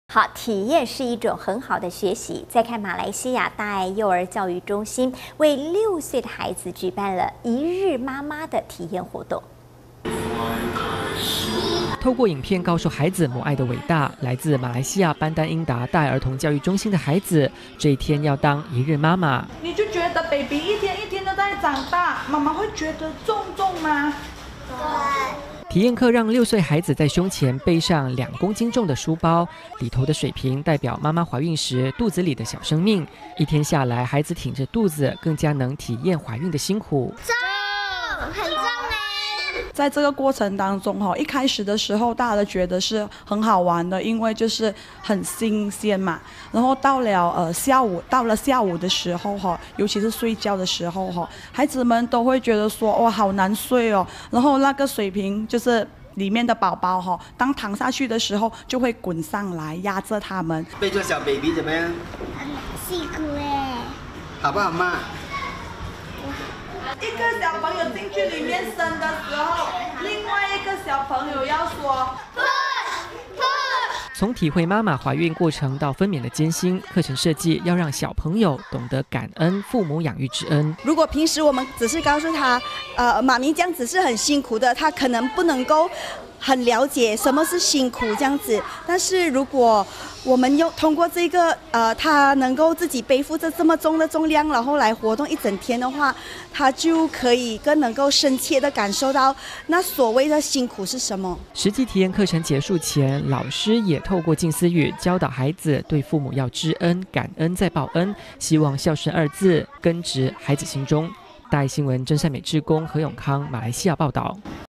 20160717【教育】一日媽媽！小朋友體會懷胎辛勞
馬來西亞班丹英達大愛兒童教育中心，為六歲班孩子舉辦【一日媽媽】體驗課。二十三位小朋友，胸前背著二公斤重的書包，體驗媽媽懷孕的辛勞，藉此讓孩子更懂得感恩親恩。

透過影片告訴孩子母愛的偉大。來自馬來西亞班丹英達大愛兒童教育中心的孩子，這一天要當一日媽媽。

「你就覺得寶寶一天一天的在長大。」

「媽媽會覺得重重嗎 ？(會) 」

體驗課讓六歲孩子在胸前背上二公斤重書包，裡頭的水瓶代表媽媽懷孕時，肚子裡的小生命。一天下來，孩子挺著肚子，更加能體驗懷孕的辛苦。

老師 陳盈智：「在這個過程當中，一開始時候大家都覺得很好玩，因為很新鮮，到了下午的時候，孩子們都會覺得好難睡，那個水瓶裡面的寶寶，當躺下去的時候，就會滾上來壓著他們。」

學生 黃月芊：「(背著小寶寶怎麼樣) 很辛苦耶，(好不好抹) 不好。」一個小朋友進去裡面生的時候
另一個小朋友要說 (推 推)

從體會媽媽懷孕過程到分娩的艱辛，課程設計要讓小朋友懂得感恩父母養育之恩。

家長 胡愫玶：「如果平時我們只是告訴他，媽咪這樣子是很辛苦的，他可能不能夠很了解 什麼是辛苦，但是如果 我們透過這個，他能夠自己背負，這麼重的重量來活動一整天的話，他就可以更能夠深切的感受到，那所謂的辛苦是什麼 。」

實際體驗課結束前，老師也透過靜思語，教導孩子，對父母要知恩、感恩、再報恩，希望孝順二字根植孩子心中。

真善美志工何永康 馬來西亞報導